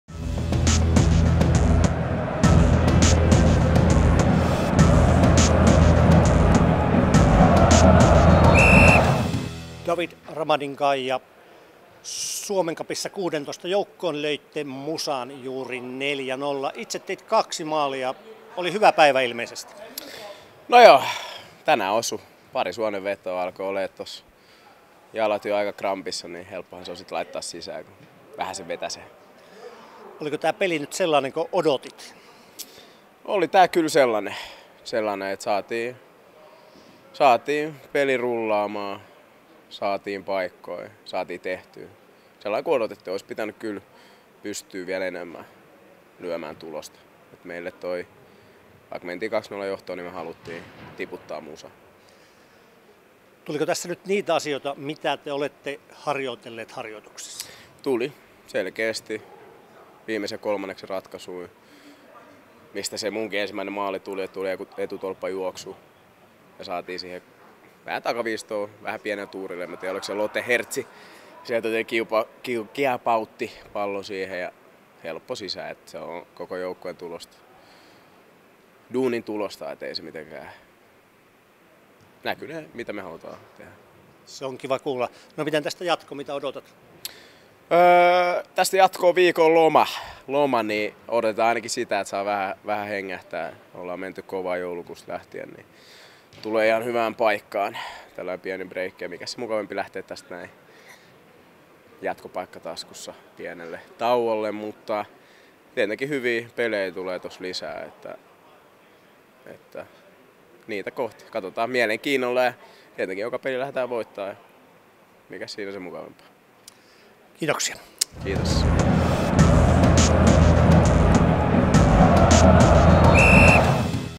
JÄLKIPELIT AC Oulu - MuSa: David Ramadingaye, AC Oulu
Ykkösen Suomen Cupin välierän jälkitunnelmia 2.3 Oulussa.

Toimittaja: Jukka Harmaala
Kuvaaja: Janne Dahlman
Leikkaaja: Janne Dahlman